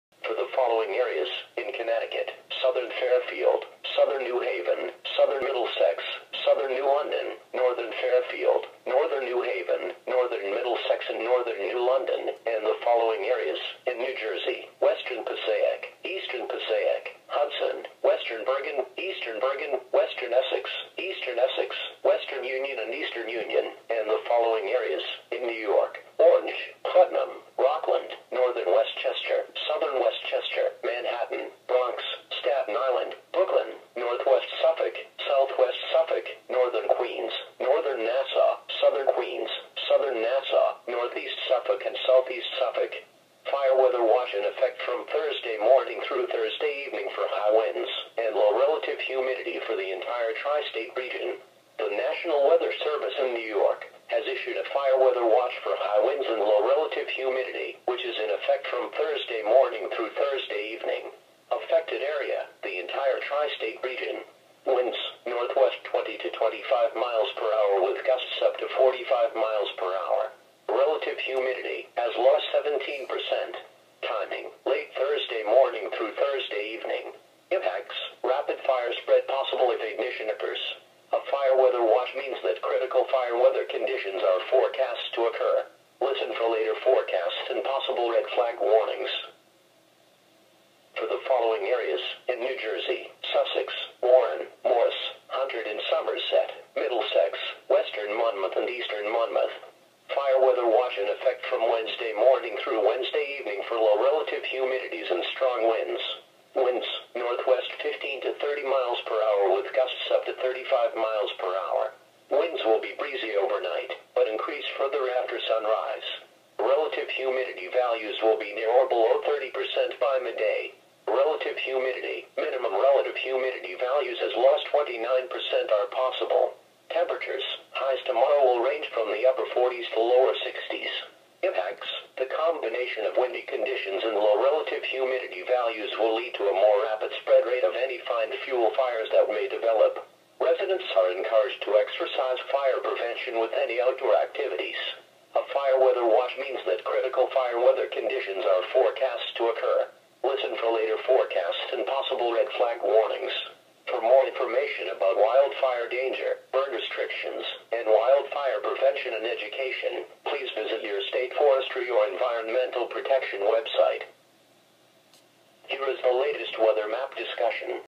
2 Fire Weather Watches for ALL of Mount Holly NJ's and Upton NY's CWAs 4-22-2014
Station: KWO35
WFO: Mount Holly, NJ and Upton, NY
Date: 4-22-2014
Text Link: N/A